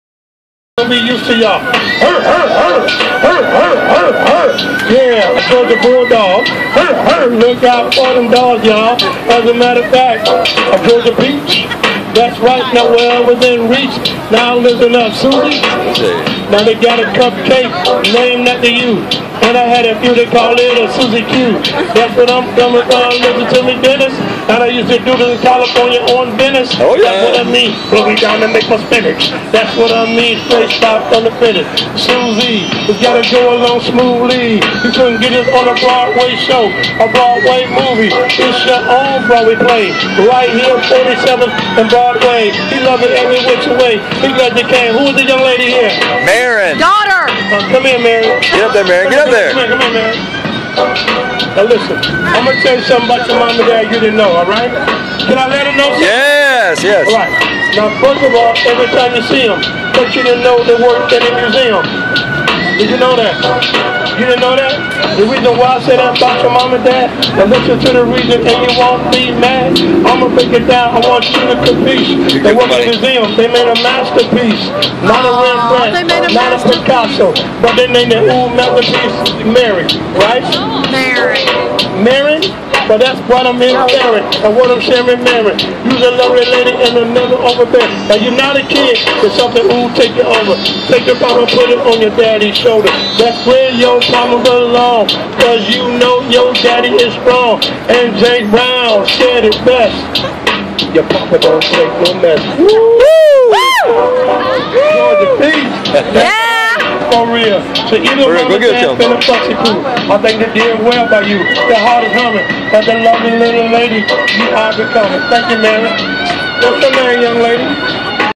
Song for Susie! Happy Mother's Day.
Commissioned at Midnight in Times Square, NYC. For Susie McBride